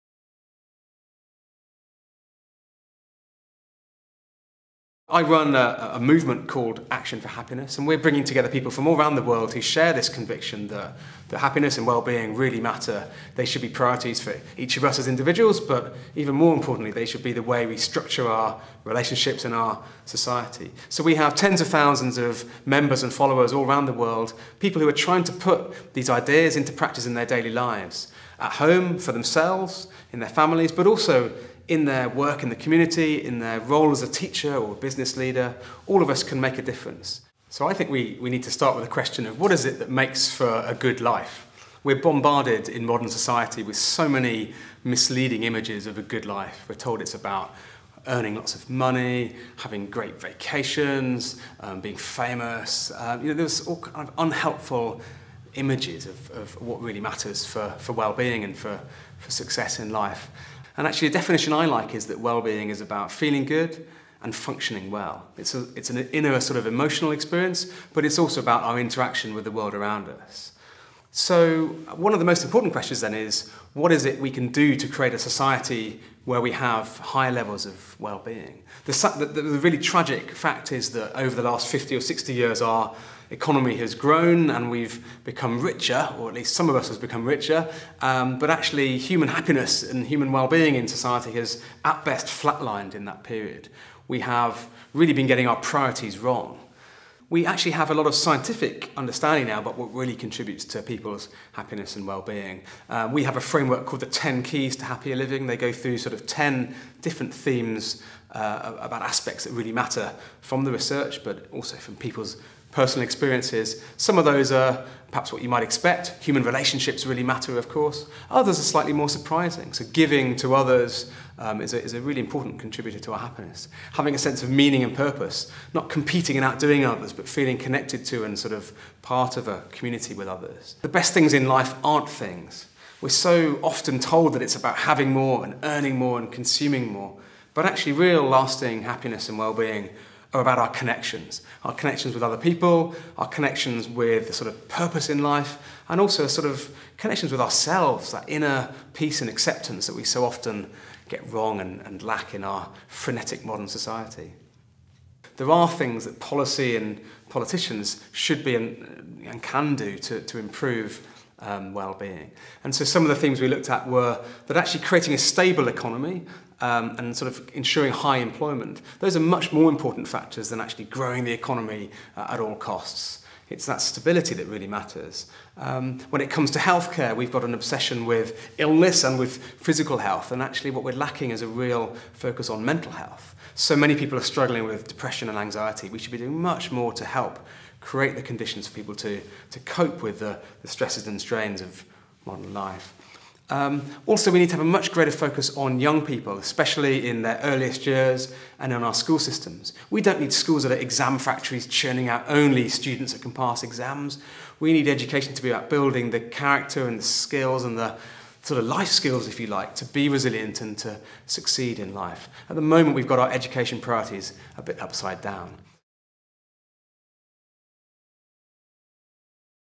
An interview with Mark Williamson, Action for Happiness
At the Festival of Wellbeing in London 2014, Network of Wellbeing spoke with Mark Williamson and his work with Action for Happiness.

An Opus Earth Production for Network of Wellbeing and Resurgence Magazine.